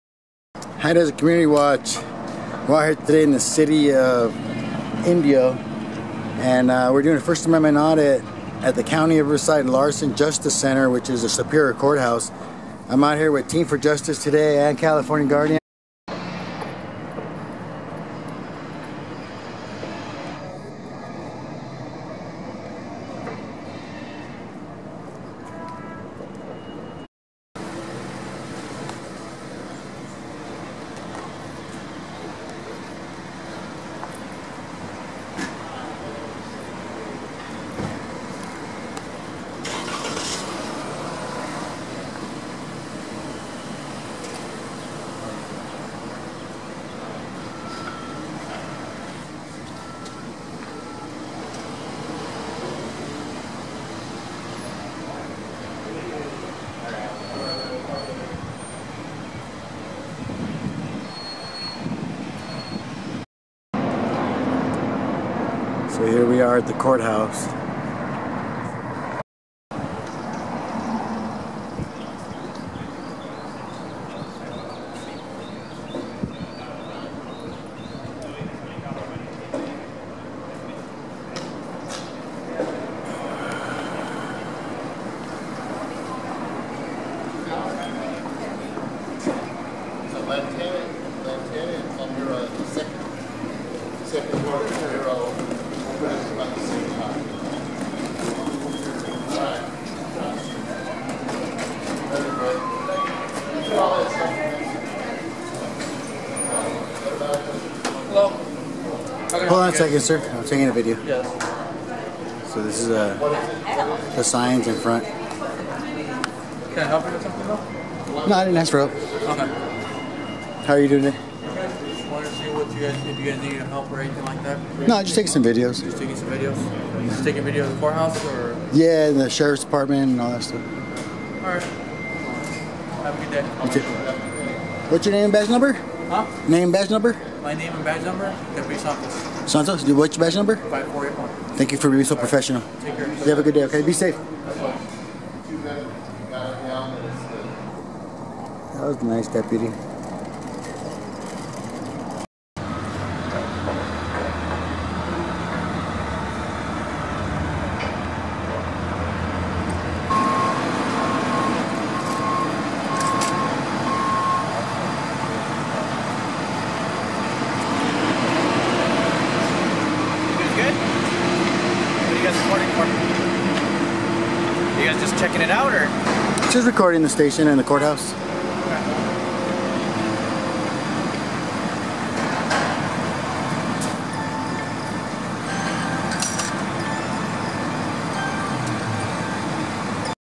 INDIO SUPERIOR COURTHOUSE AND JAIL, WHAT ARE YOU RECORDING? 1ST Amend Audit
HELP KEEP HDCW ON THE ROAD